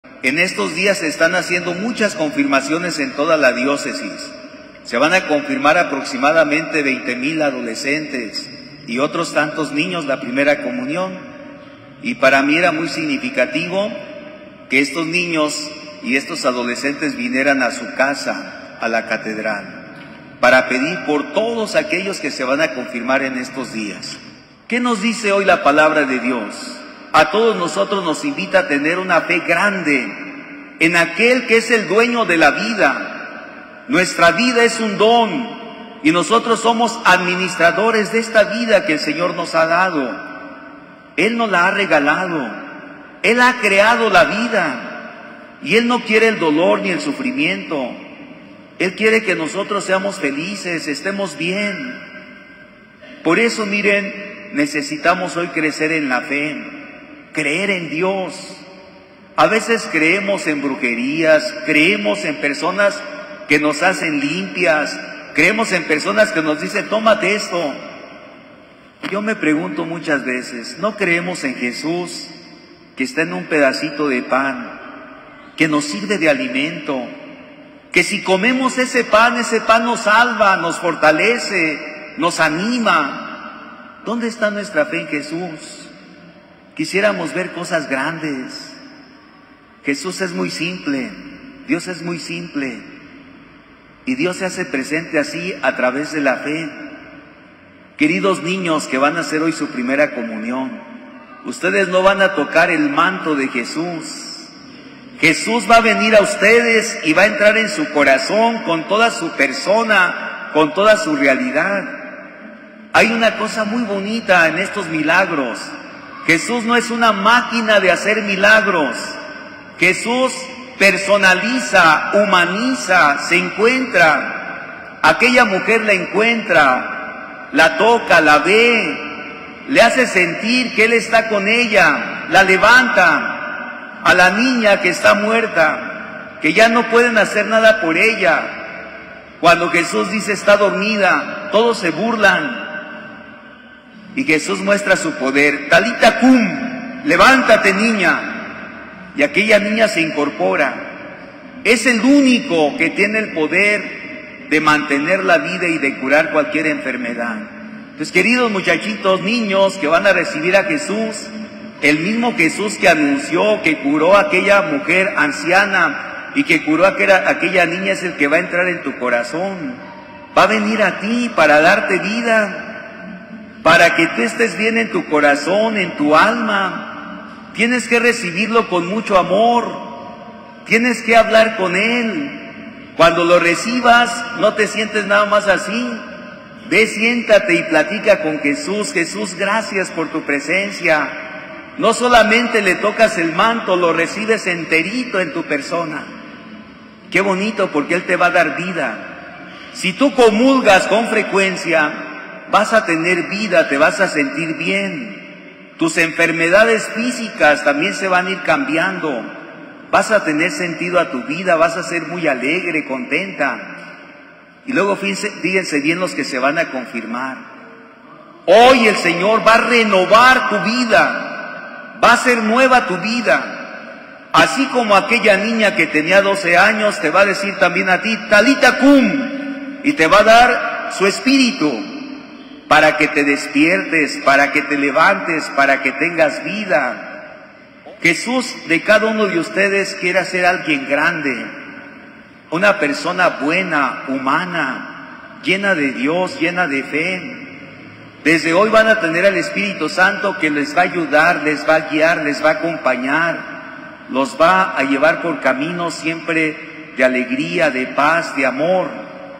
20 mil adolescentes fueron confirmados en la Fe Católica. Otros 20 mil hicieron su Primera Comunión
Más de 20 mil adolescentes de la Diócesis fueron confirmados en la Fe Católica. Otros 20 mil hicieron su Primera Comunión

* Dios es el dueño de la vida, Él nos la ha regalado y nosotros solo somos administradores: Obispo Juan Espinoza
* Muchos creen en brujerías, en personas que hacen limpias o dan brebajes. Necesitamos fortalecer nuestra fe en Jesús

Aguascalientes, Ags. 30 de junio 2024.- Durante las últimas semanas más de 20 mil adolescentes de la Diócesis han sido confirmados en la Fe Católica, en tanto que otros 20 mil han realizado su Primera Comunión, dio a conocer el Obispo Juan Espinoza Jiménez.
Durante la Misa Dominical que encabezó en la Catedral Basílica de Aguascalientes y en la cual administró los sacramentos de la Confirmación y Primera Comunión, el Pastor de la Diócesis se congratuló que los niños y adolescentes estén acercándose a la Iglesia.
A estos menores les recordó que Jesús no es una máquina de hacer milagros, sino que es el dueño de la vida, don que nos ha sido regalado y del cual solo somos administradores.
Pidió crecer en la fe, creer en Dios y no en brujerías, en personas que hacen limpias o dan brebajes. Hoy, dijo, debemos reencontrar nuestra fe en Jesús.